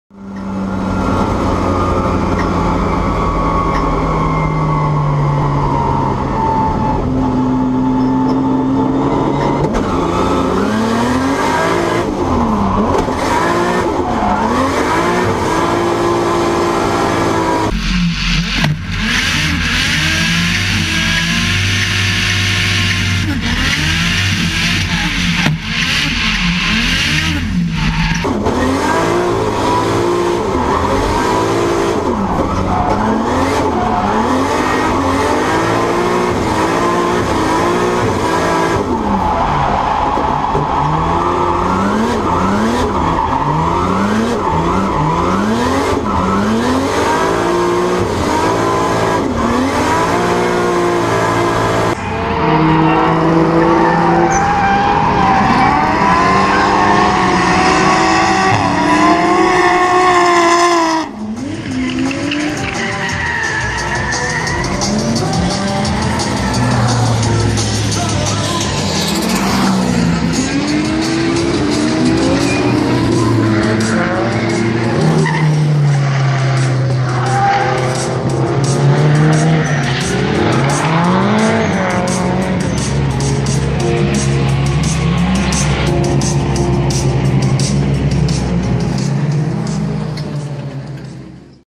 Jacob Hakaraia - Zeroclass Winter Drift Matsuri 2015 Taupo
D1NZ Driver - Jacob Hakaraia from Hamilton on form at Zeroclass Winter Drift Matsuri event at Taupo NZ

Driving his C35 Laurel with the 2JZ motor and trialling the new Nexen 265/35/18 USA SUR4 Semi slicks on the rear it was a weapon of mass angle and smoke!

A successful day at an awesome event!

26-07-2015